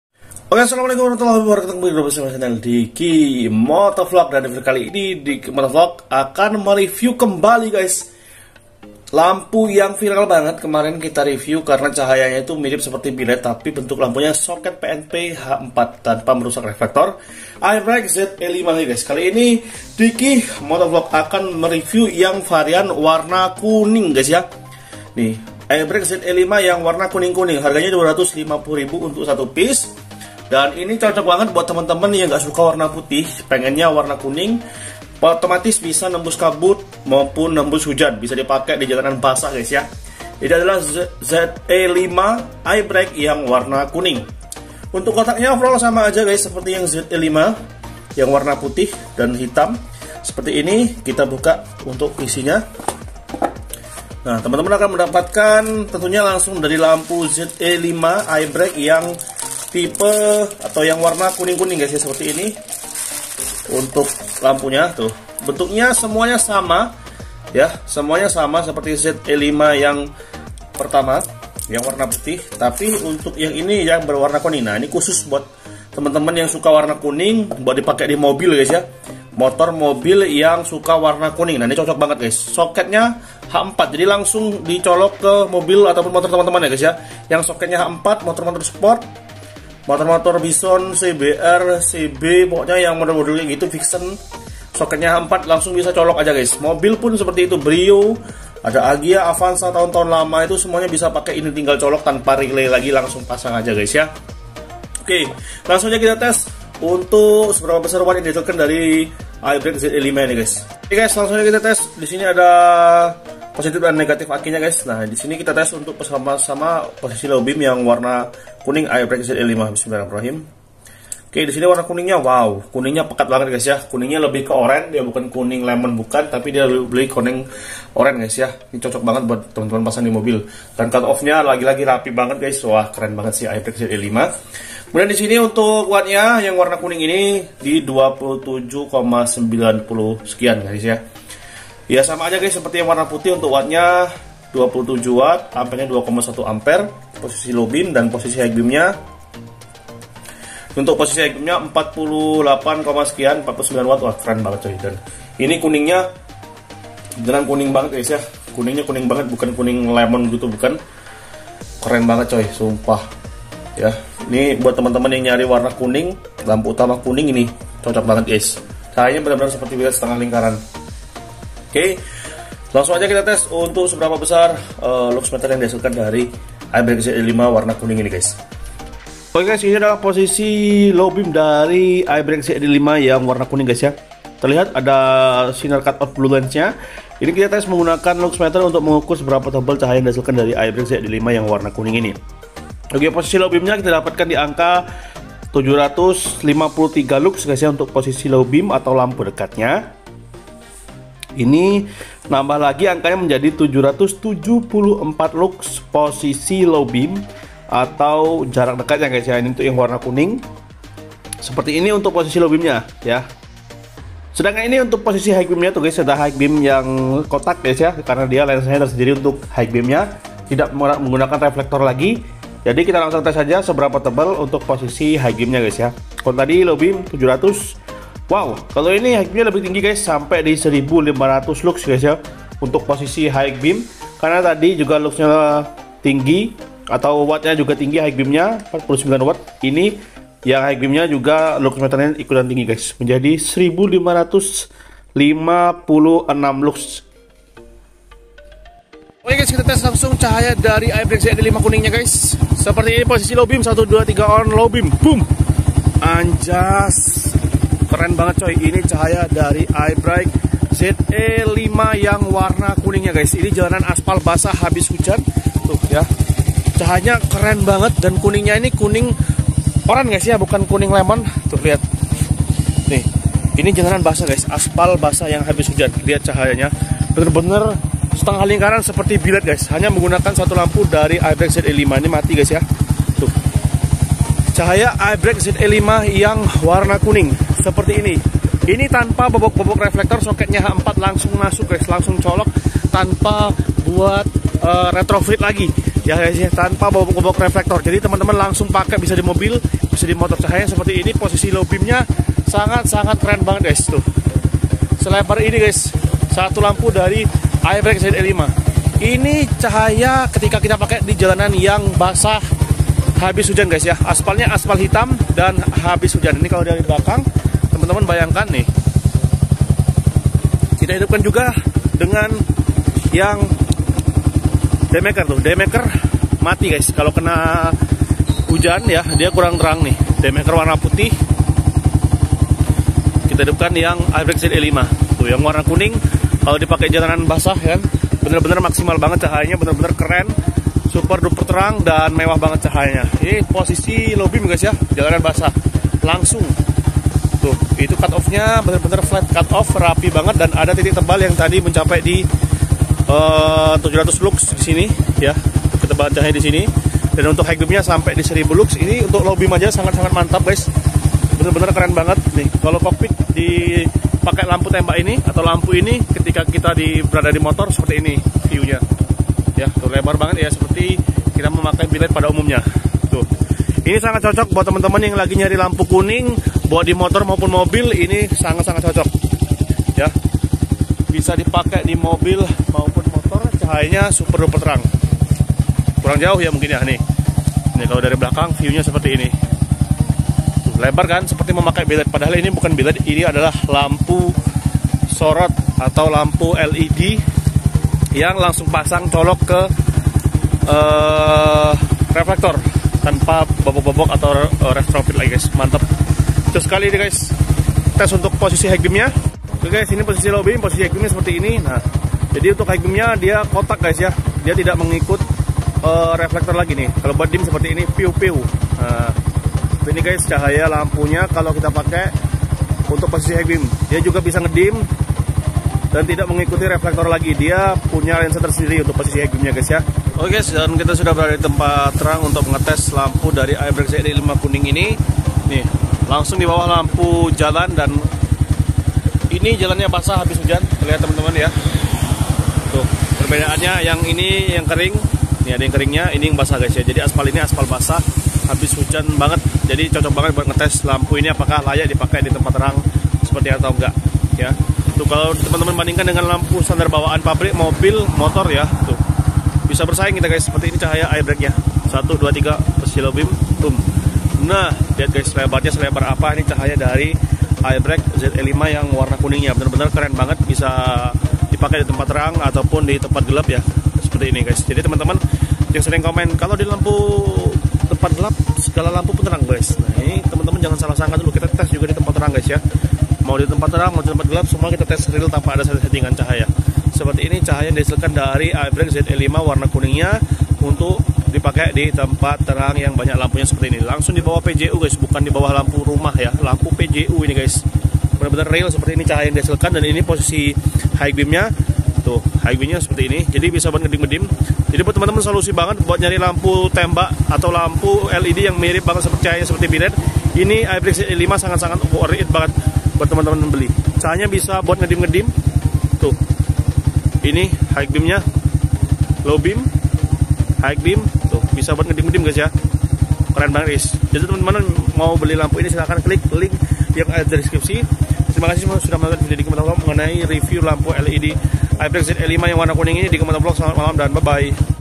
IBRIGHT ZE 5 KUNING - LAMPU UTAMA TEMBUS KABUT & HUJAN !
Berikut video review lampu lampu utama yang cocok untuk tembus kabut dan hujan yaitu ibright ze 5 warna kuning langsung pnp tanpa bobok reflektor.

YANG MAU BELI LANGSUNG KE LINK DIBAWAH 👇👇👇👇👇👇👇👇👇?

KOLEKSI LAMPU TEMBAK DIKI MOTOVLOG
BELINYA DISINI YA GUYS